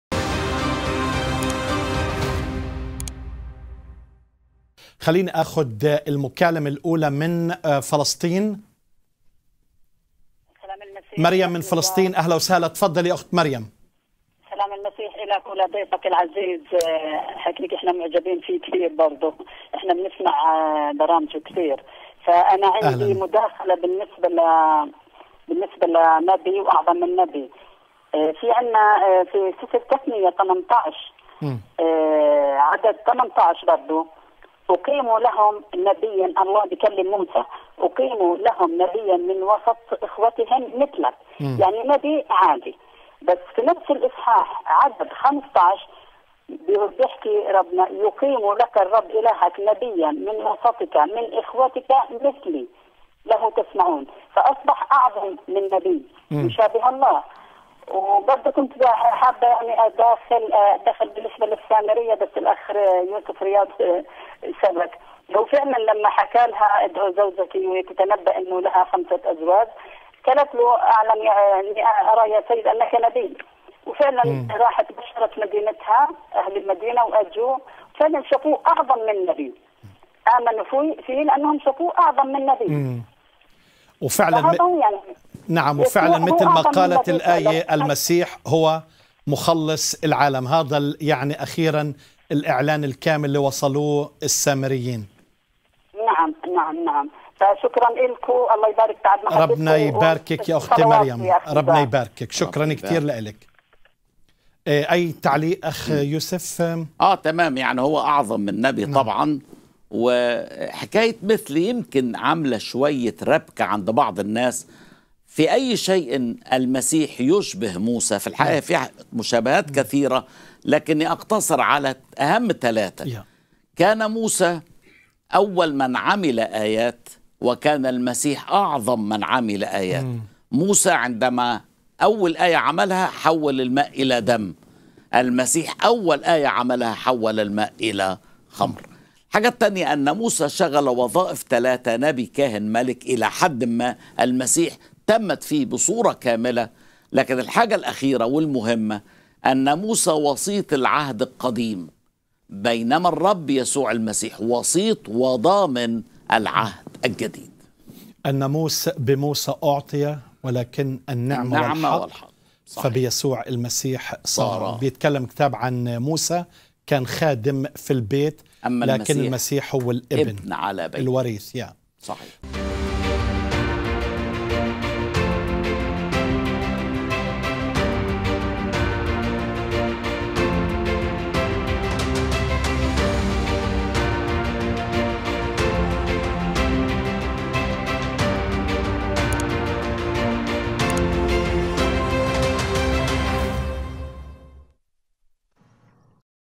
448 مقارنة بين موسى والمسيح
عزيزي المشاهد
هذه الحلقة بعنوان ( هل المسيح نبي أم أعظم ؟ )
ضيف هذه الحلقة خادم الرب الأخ/ يوسف رياض
يمكنك مشاهدة الحلقة كاملة بالضغط على هذا الرابط: